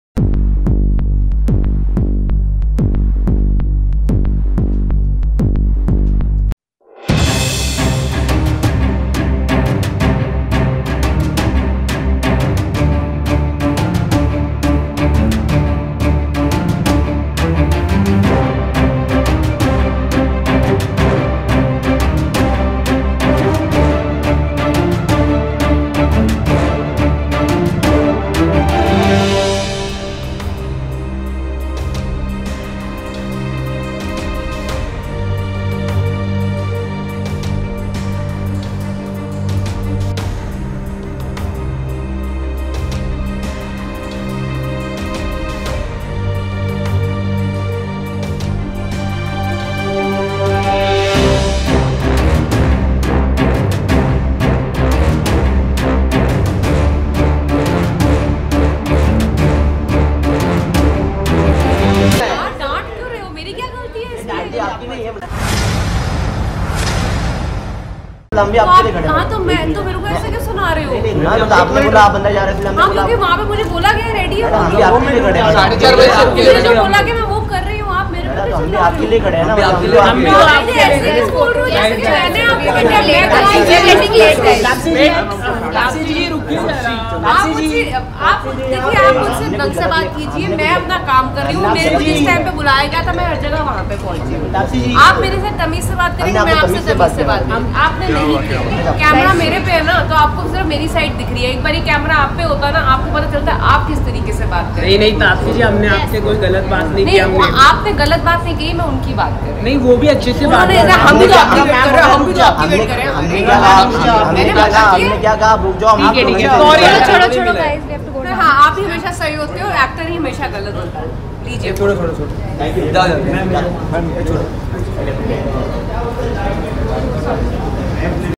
Tapsee Pannu Had A Big Fight👿With Media When Media Why Se Got Late@ Song Launch Of Do Baaraa
Tapsee Pannu Had A Big Fight👿With Media When Media Why Se Got Late@ Song Launch Of Do Baara